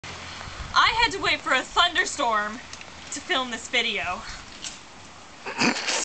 Emily Irving New Moon
this is a part of the contest video that had to get cut because it was only aloud to be 60 seconds long ! plus you can hear my laugh at the end of it hahaha i laugh everytime and have to stop recording